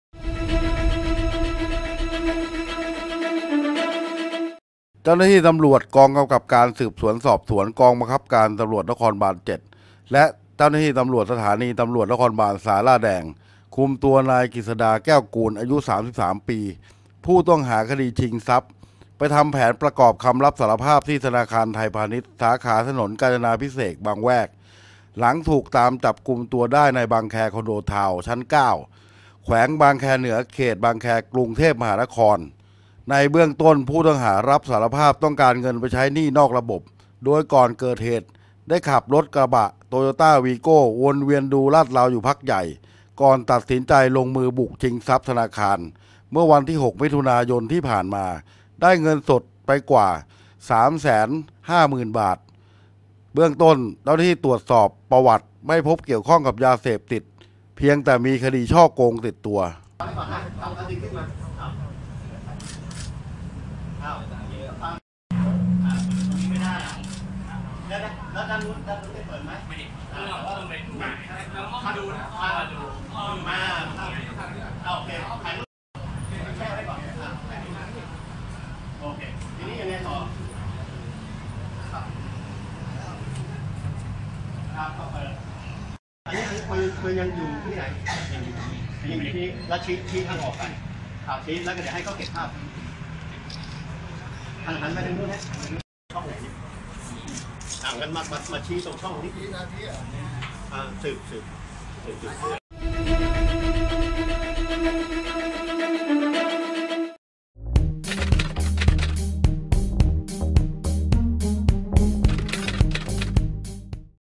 จับคนร้ายชิงทรัพย์ ธ.ไทยพาณิชย์ทำแผน | เดลิ[HOT]นิวส์ 080661
ตำรวจนครบาลศาลาแดง คุมตัวนายกฤษฎา แก้วกูล ผู้ต้องหาคดีชิงทรัพย์ธนาคารไทยพาณิชย์ ไปทำแผนประกอบคำรับสารภาพ หลังถูกจับกุมได้ที่บางแคคอนโดทาวน์ เขตบางแค